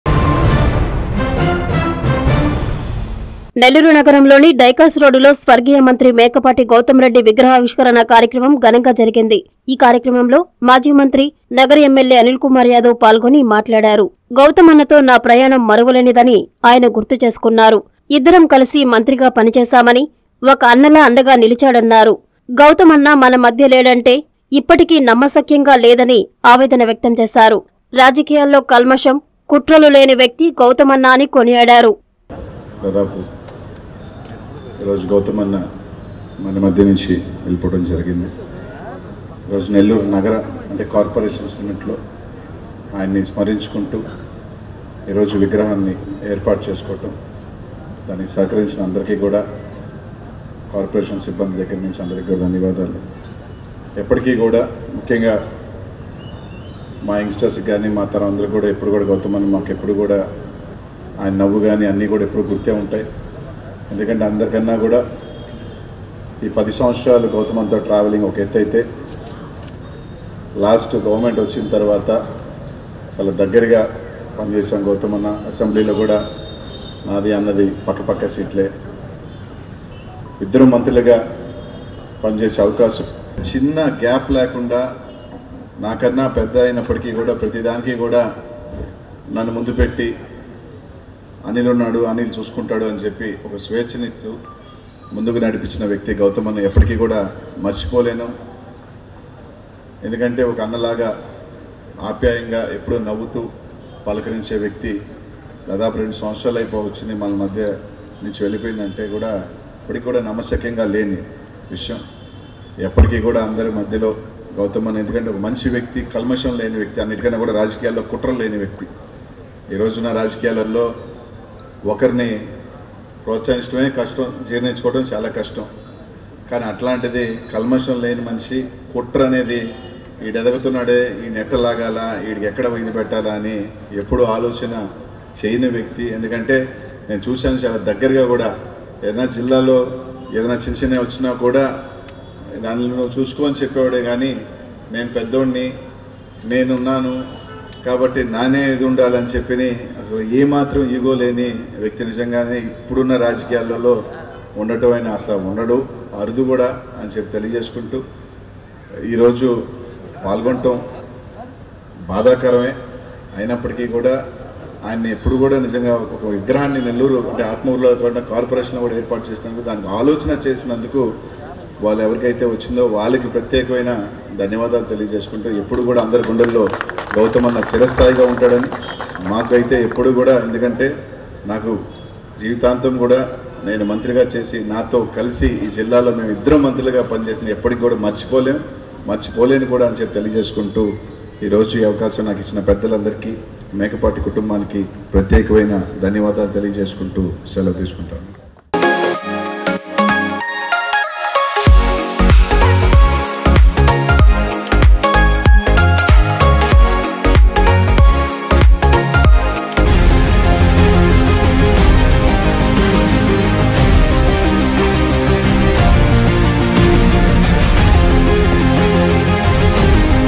అన్న‌లా...అండ‌గా నిలిచాడు | MLA Anil Kumar Yadav about Mekapati Goutham Reddy | Nellore
- గౌత‌మ్ అన్న లేడంటే ఇప్ప‌టికి న‌మ్మ‌లేక‌పోతున్నా
- స్వ‌ర్గీయ మేక‌పాటి కాంస్య విగ్ర‌హ ఆవిష్క‌ర‌ణ‌లో...ఎమ్మెల్యే అనిల్